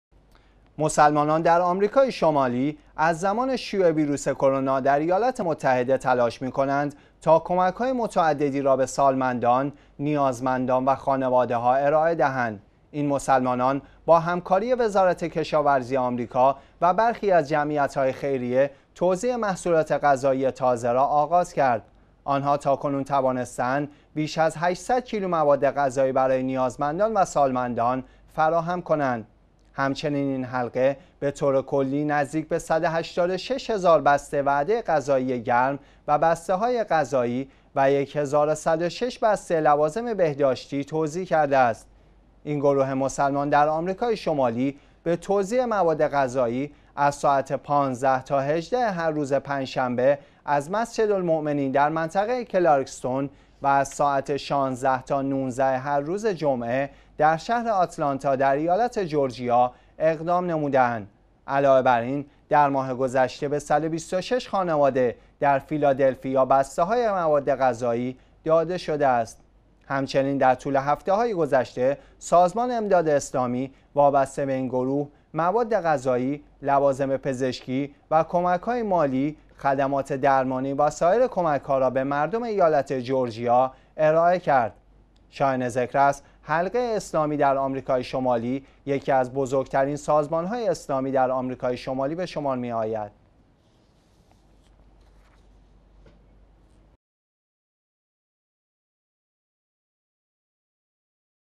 تلاش مسلمانان برای راه اندازی کمپین کمک به نیازمندان آمریکا
مسلمانان در آمریکای شمالی از زمان شیوع ویروس کرونا در ایالات متحده، تلاش می کنند تا کمک های متعددی را به سالمندان، نیازمندان و خانواده ها ارایه دهند.
این مسلمانان، با همکاری وزارت کشاورزی آمریکا و برخی از جمعیت های خیریه توزیع محصولات غذایی تازه را آغاز کرد.
آنها تاکنون توانسته اند بیش از 800 کیلو مواد غذایی برای نیازمندان و سالمندان فراهم کنند. همچنین این حلقه به طور کلی نزدیک به 186 هزار وعده غذایی گرم و بسته های غذایی و یک هزار و 106 بسته لوازم بهداشتی توزیع کرده است.
این گروه مسلمان در آمریکای شمالی به توزیع مواد غذایی از ساعت 15 تا 18 هر روز پنجشنبه از مسجد المومنین در منطقه کلارکستون و از ساعت 16  تا 19 هر روز جمعه در شهر اتلانتا در ایالت جورجیا اقدام نموده اند.
علاوه براین، در ماه گذشته به 126 خانواده در فیلادلفیا بسته های مواد غذایی داده شده است.
همچنین در طول هفته های گذشته سازمان امداد اسلامی وابسته به این گروه، مواد غذایی، لوازم پزشکی و کمک های مالی، خدمات درمانی و سایر کمک ها را به مردم ایالت جورجیا ارایه کرد.
شایان ذکر است، حلقه اسلامی در آمریکای شمالی (ICNA) یکی از بزرگ ترین سازمان های اسلامی در آمریکای شمالی به شمار می آید.